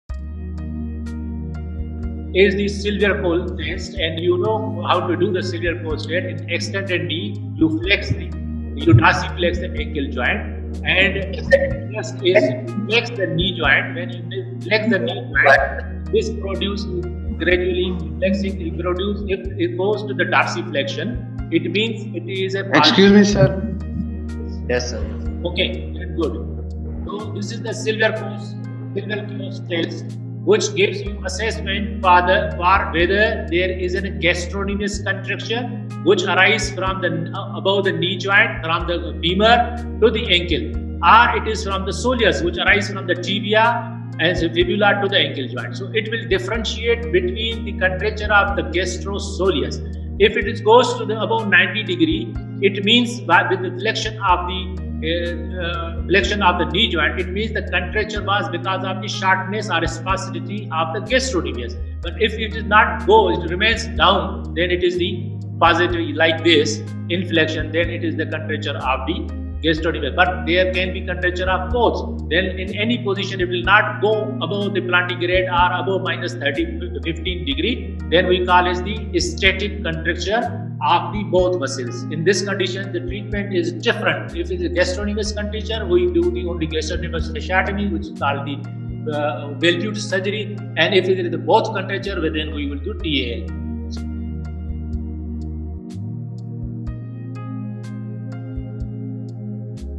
Silfverskiöld Test Explained – Gastrocnemius vs. Soleus Contracture in Cerebral Palsy Lecture
📌 Silfverskiöld Test Explained – Gastrocnemius vs. Soleus Contracture in Cerebral Palsy . A Lecture by Prof. Dr. Anisuddin Bhatti . Also known as the Achillis Stretch Test.

In this orthopedic-focused clinical lecture, we demonstrate the Silfverskiöld Test, a crucial physical exam maneuver used to differentiate between gastrocnemius and soleus muscle contractures in children with equinus deformity, often seen in cerebral palsy.

Understanding the biomechanics behind the ankle dorsiflexion limitation is essential for planning effective interventions, including botulinum toxin injections, orthotic management, and surgical procedures such as gastrocnemius recession or tendo-Achilles lengthening (TAL).

🔎 What You’ll Learn:

How to perform the Silfverskiöld Test step-by-step

Difference between isolated gastrocnemius and combined gastro-soleus contracture

How to interpret test findings for treatment planning

Relevance in spastic cerebral palsy, toe walking, and clubfoot relapse

🎯 Best For:
✔️ Pediatric Orthopedic Surgeons
✔️ Pediatricians
✔️ Physiotherapists & Rehab Experts
✔️ Orthopedic Residents & Medical Students

🧠 Mastering this test is key to diagnosing the type of equinus deformity, guiding whether a child needs conservative management or surgical correction.